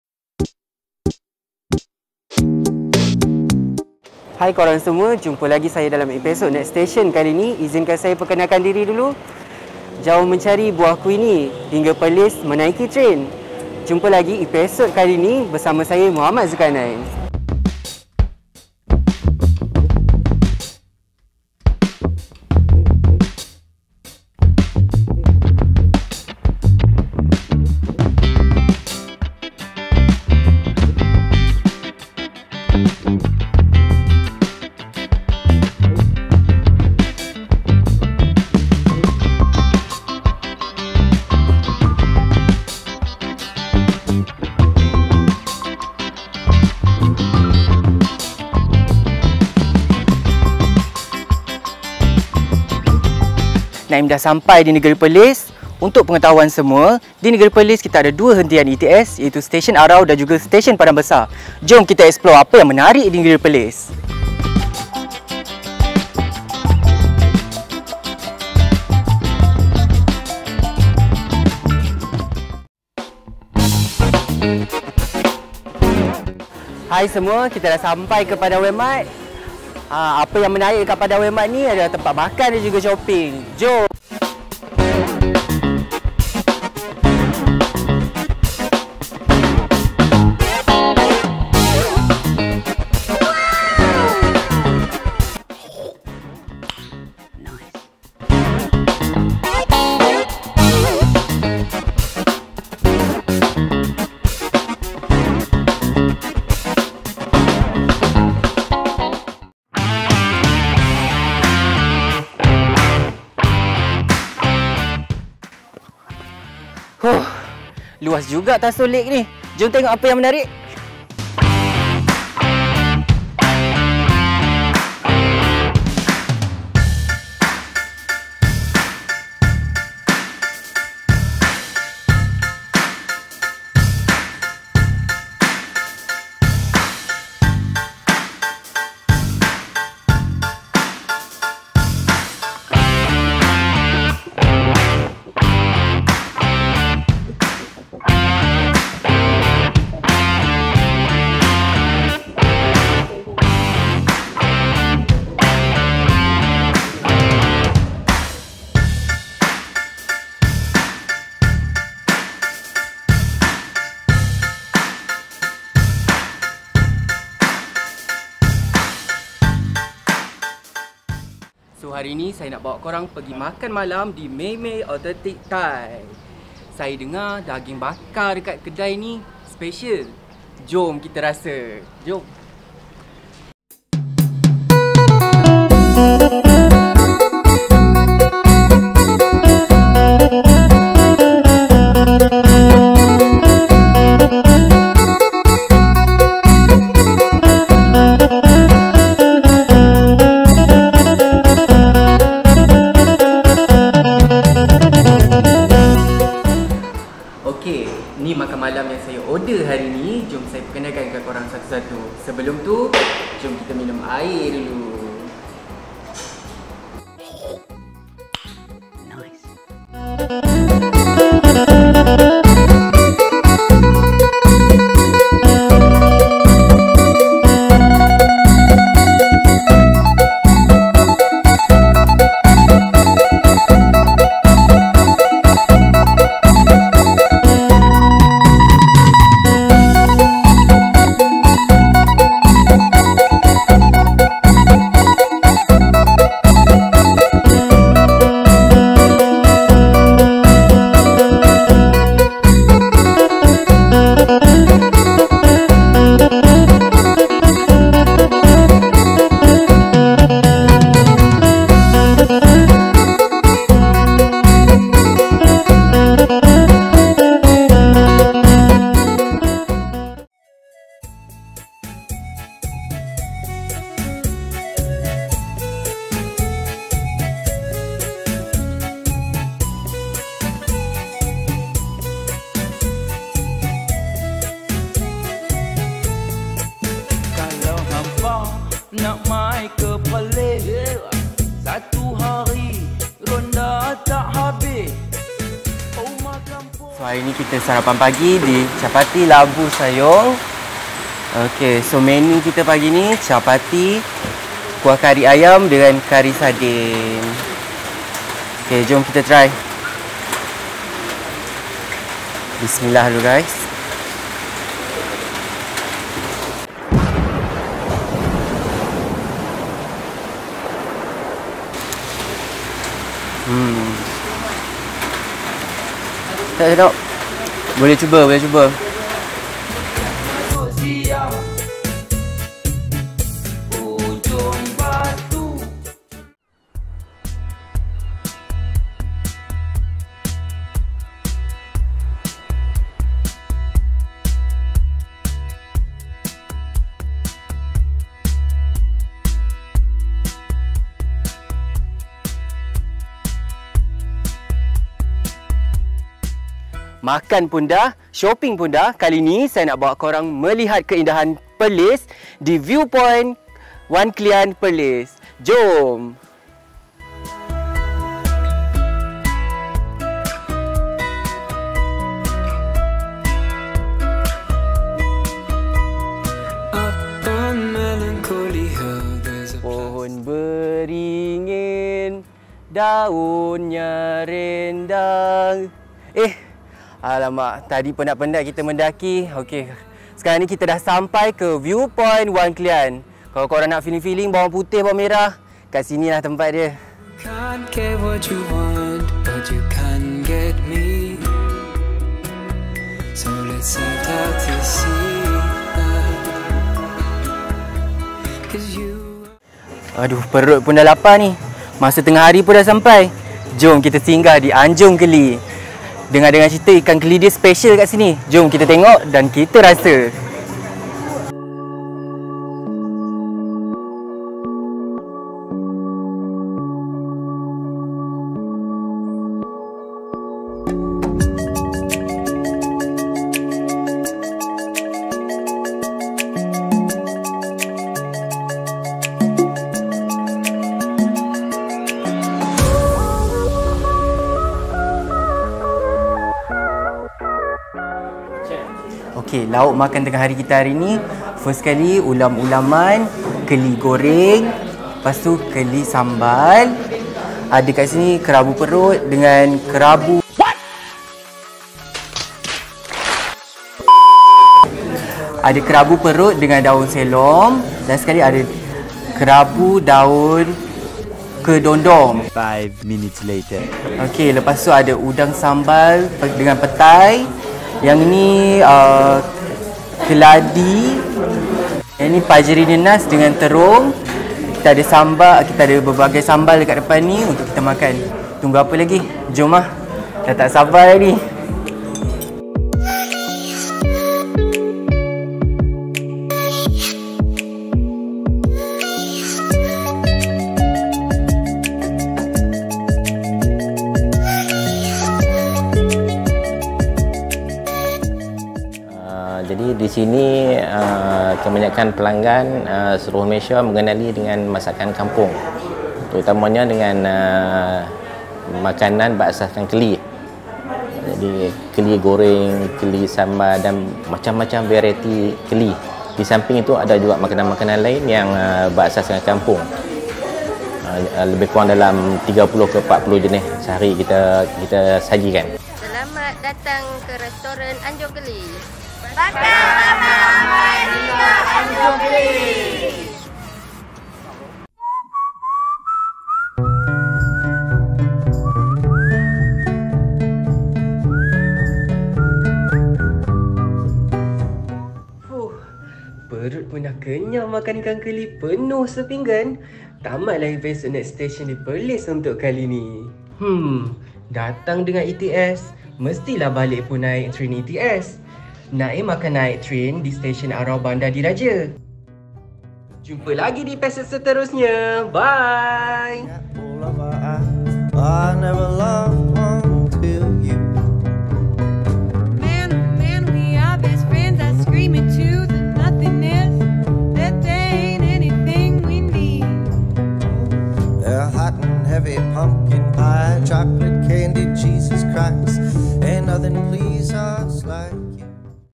Next Station: Mengembara Peghelih menaiki tren ETS
Cuti tahunan korang banyak lagi tak? kalau banyak jom min nak suggest tempat menarik untuk korang pergi.

Min nak pi negeri yang dekat dengan Thailand, nak feeling-feeling pi obersea.

Trip kali ini min explore Perlis gais.

Banyak tempat menarik yang boleh dilawati dan tidak lupa juga tempat makan yang best dan harga berbaloi.

Nak tau min p mana jom tonton video ni!